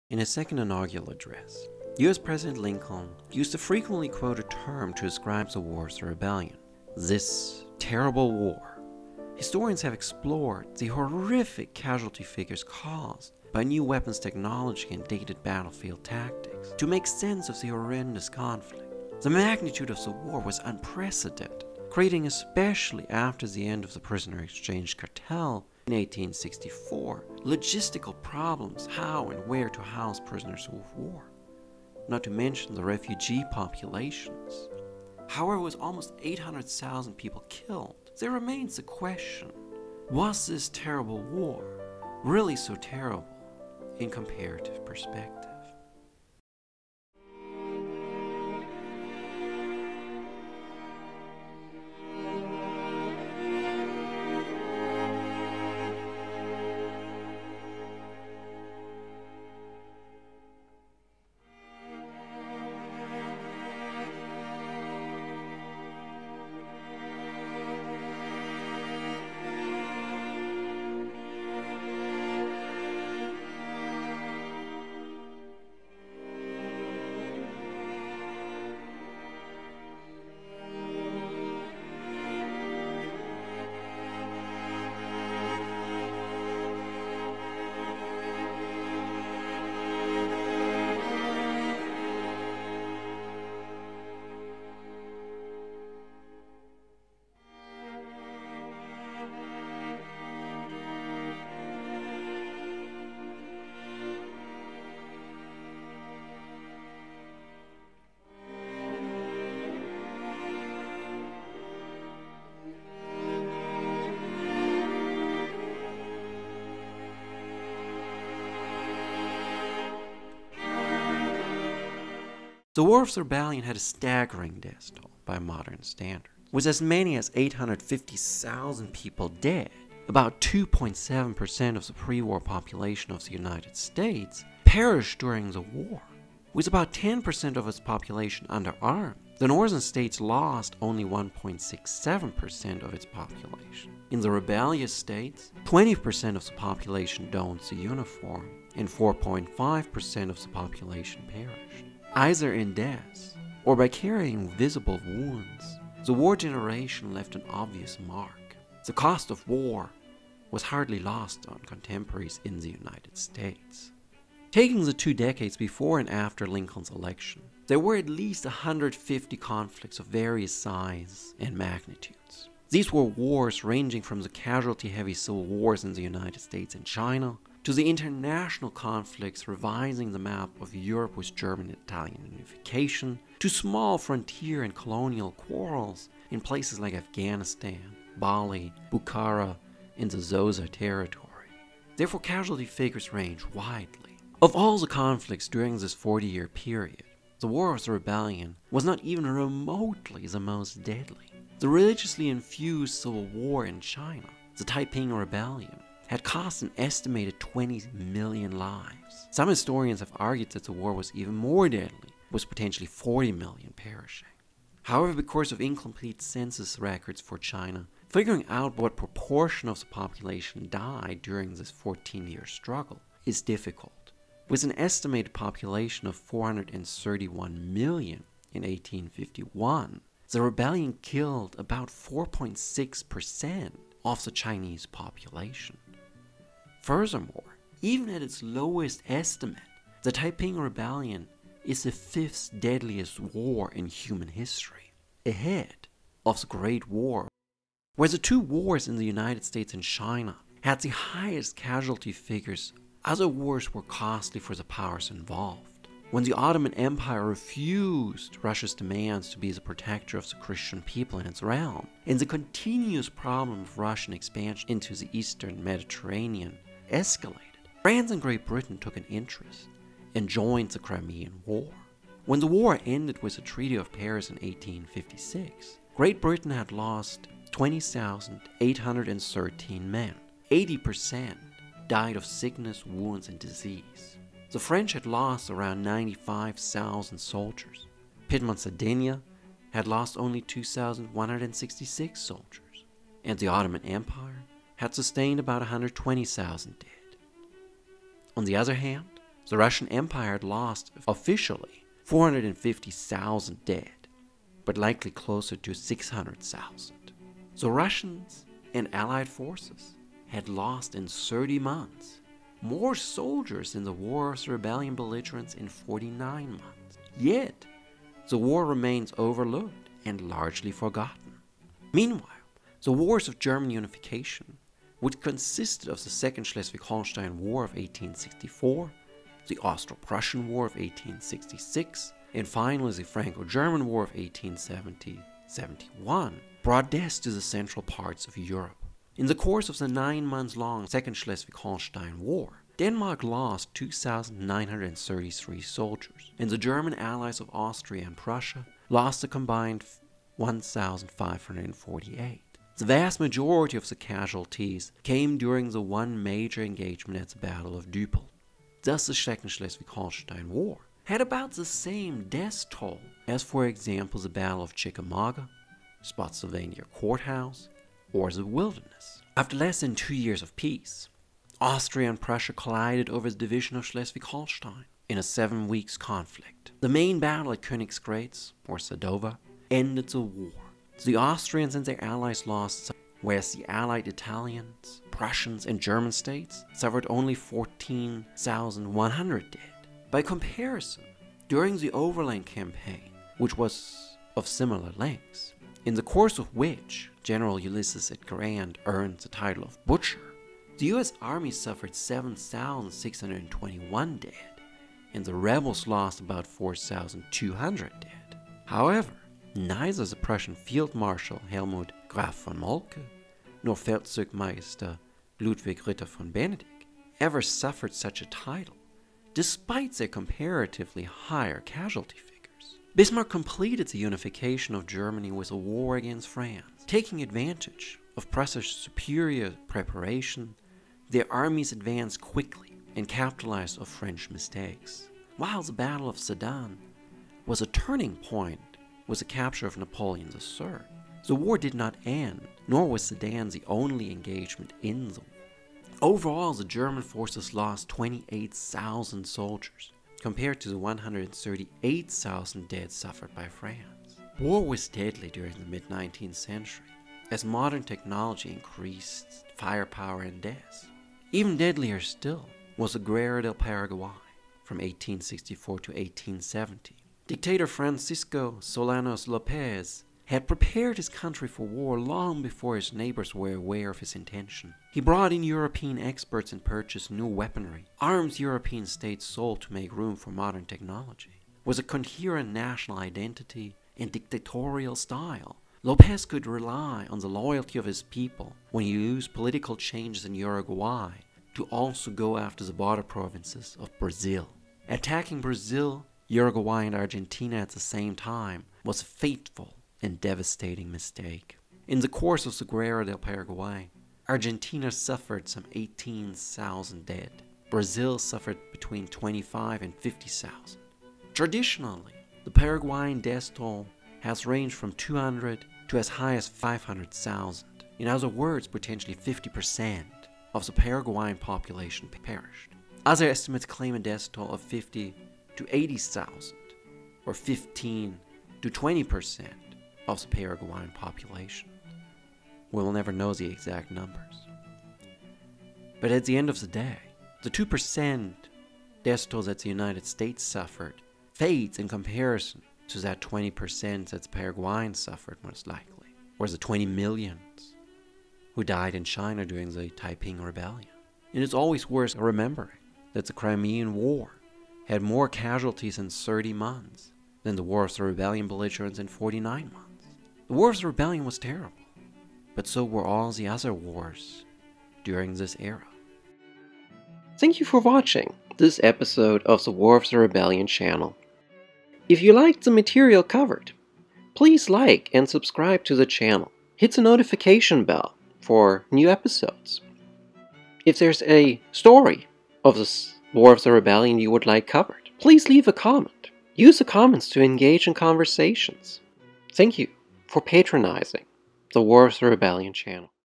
Episode 19--This Terrible War?
In his second Inaugural Address, Lincoln referred to the War of the Rebellion as "This Terrible War." In this episode, we will comparatively look at the War of the Rebellion and whether the conflict was this terrible in comparison with other wars at the time. Looking at the casualty-heavy conflicts in China and Paraguay, a different picture emerges. Even the often forgotten Crimean War was far more deadly then the War of the Rebellion.